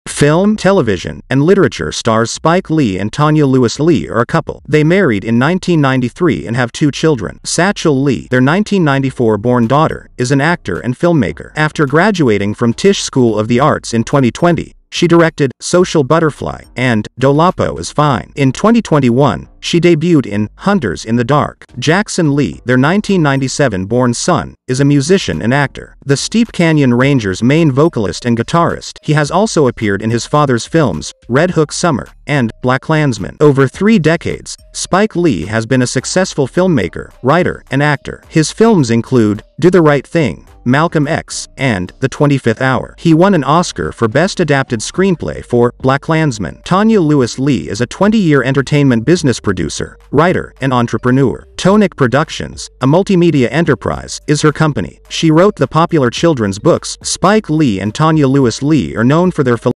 Spike Lee and Tonya Lee Lewis ❤❤❤
Spike Lee and his wife, the writer and producer Tonya Lewis Lee, are household names in the media world thanks to their many accomplishments across film, television, and literature. They tied the knot back in 1993 and are now the proud parents of two young ones.

Satchel Lee, their 1994-born daughter, is a director and actor in her own right. She has directed many short films since graduating from NYU's Tisch School of the Arts in 2020, including "Social Butterfly" and "Dolapo is Fine." She has also appeared in a number of short films and will make her feature film debut in the upcoming 2021 film "Hunters in the Dark."

Their son, the musician and actor Jackson Lee, was born in 1997. He is the band's lead singer and guitarist and has appeared in various movies, including his father's "Red Hook Summer" and "BlacKkKlansman."

Spike Lee has been working in the entertainment industry for almost 30 years, during which time he has won numerous awards for his filmmaking, writing, and acting. His most notable film roles are in "Do the Right Thing," "Malcolm X," and "The 25th Hour." One of the many accolades he has received is an Academy Award for Best Adapted Screenplay for his work on "BlacKkKlansman."

Tonya Lewis Lee has been a successful producer, writer, and businesswoman in the film and television industry for almost two decades. She created the media conglomerate ToniK Productions and serves as its chief executive officer. She has penned several books for kids, including the best-sellers "Please, Baby, Please" and "Please, Puppy, Please."

Spike and Tonya Lewis Lee have been honored jointly for their philanthropic and creative work in the film business. They've done work for the NAACP Legal Defense Fund, the Children's Health Fund, and the New York Public Library, among others.

Together, Spike Lee and his wife, Tonya Lewis Lee, are a creative powerhouse that has made waves in the film world and beyond. Satchel and Jackson, their offspring, have followed in their footsteps and established themselves as leaders in the field.